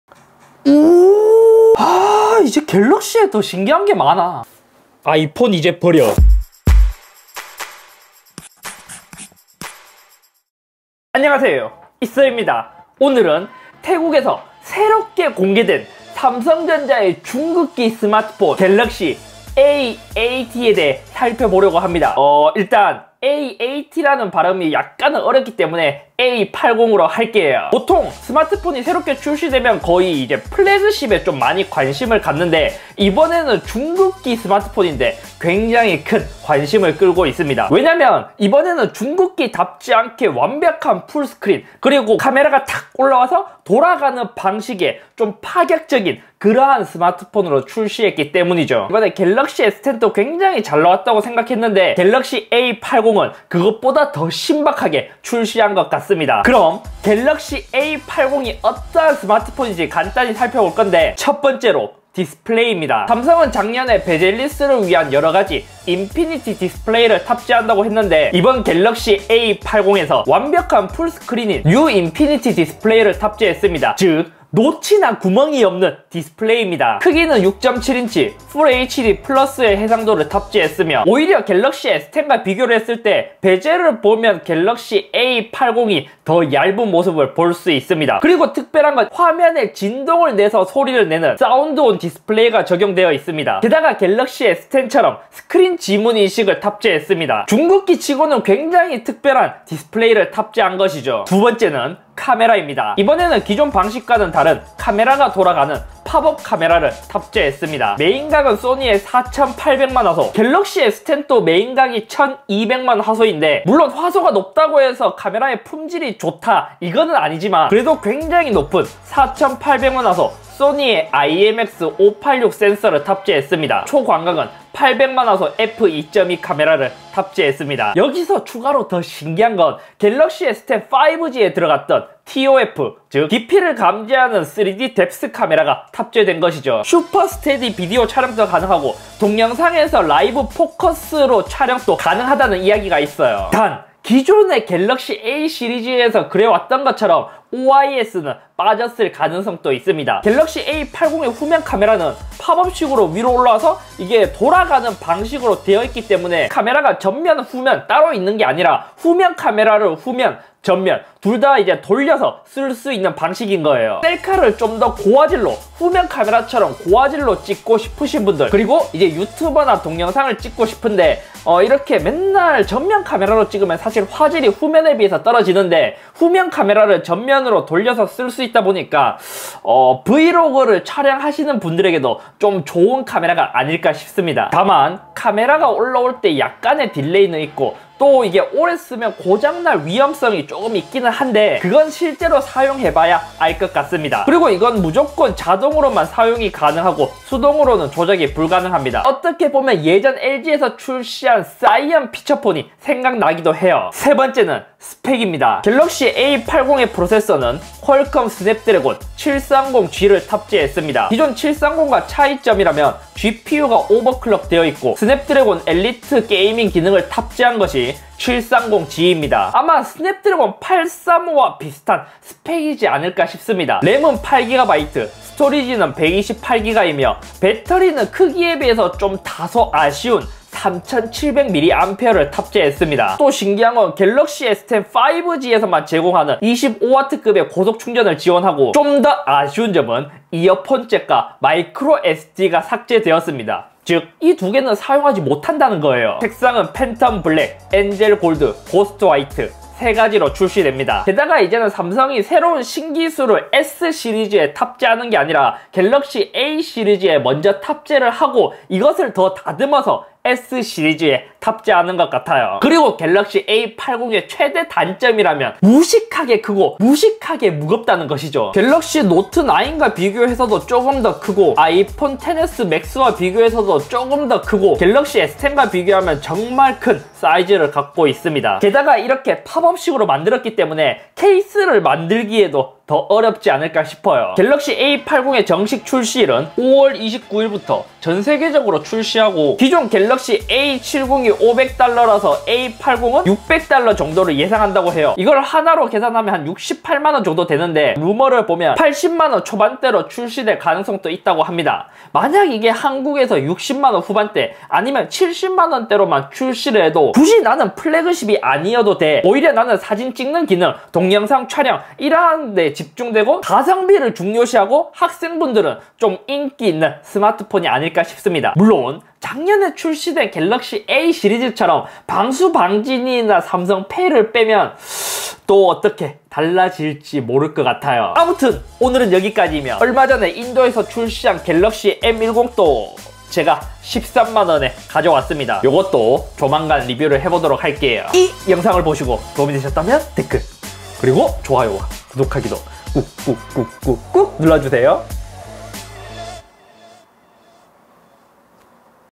진짜 카메라가 돌아버린 삼성 갤럭시 A80 공개 총정리! 노치와 구멍도 없는 풀 스크린 스마트폰
금일 삼성전자의 중가형 스마트폰인 갤럭시 A80이 새롭게 공개되었습니다. 이번에는 기존과 더욱 다르게 팝업식 로테이션 카메라를 탑재했는데, 생각보다 갤럭시 S10에서 들어간 기능보다 더 좋은것도 있고 못한것도있고 파격적인 스마트폰이라 생각됩니다.

★ Spec.
 모델명 : 삼성 갤럭시 A80
 프로세서 : 퀄컴 스냅드래곤 730G
 RAM : 8GB
 STORAGE : 128GB
 디스플레이 : 6.7인치 FHD+(1080x2400)
 카메라 : 메인 4800만화소 F2.0(IMX586), 초광각 800만화소 F2.2, ToF 카메라
 배터리 : 3,700mAh
 특징 : 화면 속 스피커, 디스플레이 지문인식, 25W 고속충전, 이어폰잭 제거, 마이크로 SD 미지원
 색상 : 팬텀 블랙, 엔젤 골드, 고스트 화이트
 가격 : 미정